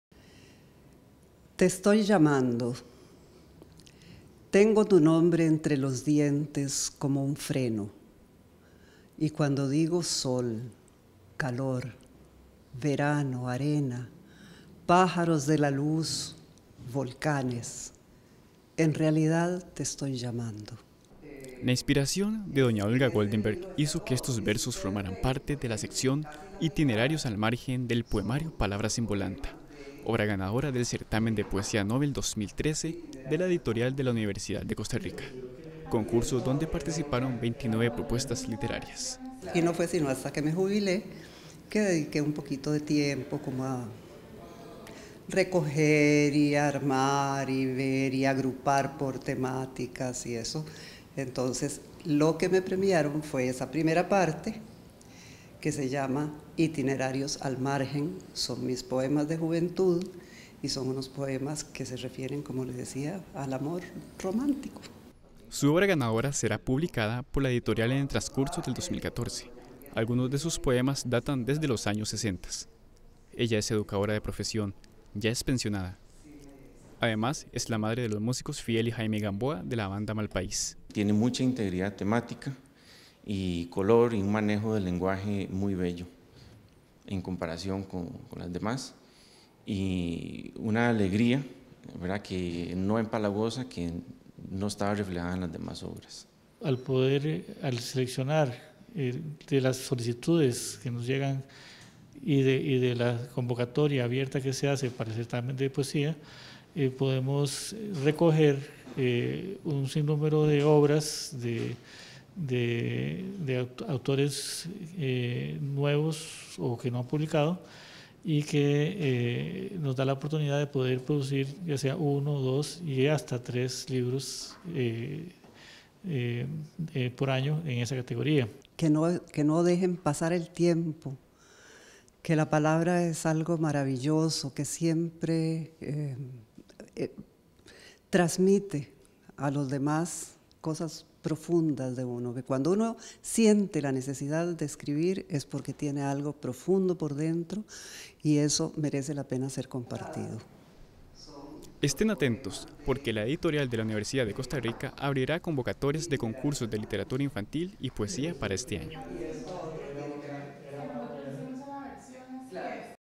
Ganadora del Certamen de Poesía Nobel 2013 de la Editorial UCR
Con el poemario "Palabras en Volanta",  Olga Goldenberg ganó el "Certamen de Poesía Novel 2013" que organiza la Editorial de la Universidad  de Costa Rica.